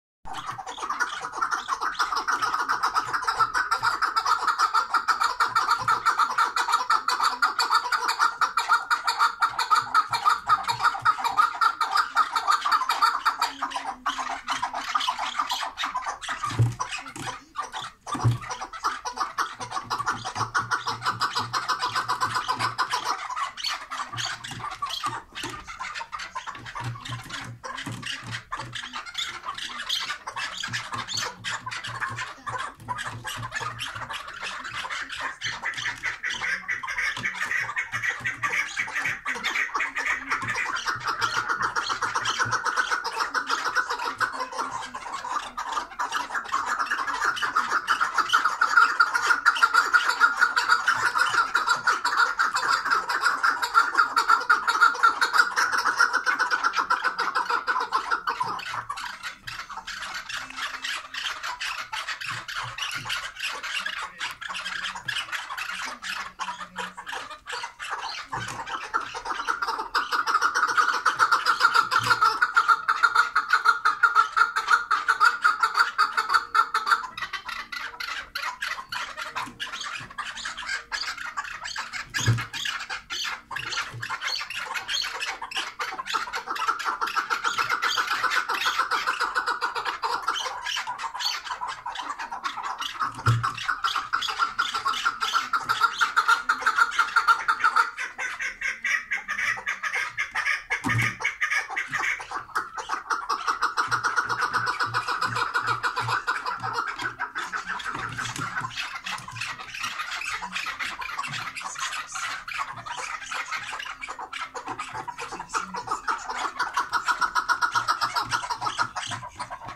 Herkes Bu Keklikleri Konuşuyor !!! Süper Keklik Sesi
Keklik Sevdalıları için yine çok güzel bir Keklik sesi ziyafeti